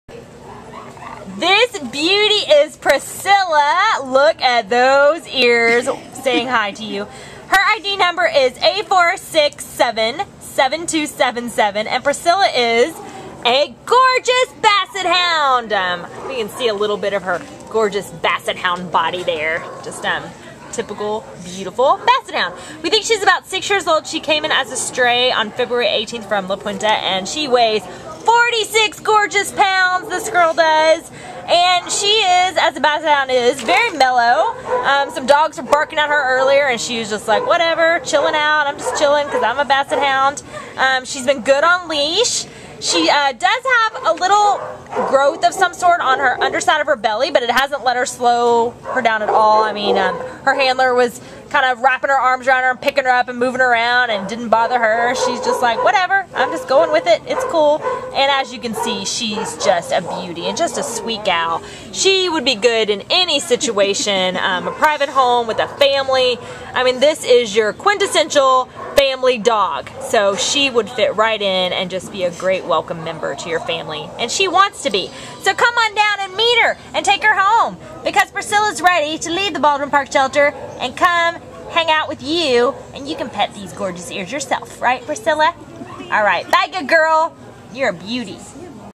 Priscilla A4677277
A4677277 Priscilla is a bombproof six-year-old tricolour female purebred Basset Hound who was found in La Puente and brought to the Baldwin Park Animal Care Center on February 18th. Weighing 46 pounds, Priscilla was clearly used as a breeding dog before she found her way to the shelter, but she remains a happy and friendly pup to everyone she meets. Chill and mellow, Priscilla walks well on leash, gets along swimmingly with other dogs, and is probably housebroken. She will make a great family dog. Priscilla will need to see a vet to examine a growth on her underbelly. She will make a fantastic indoor companion for a family in any living situation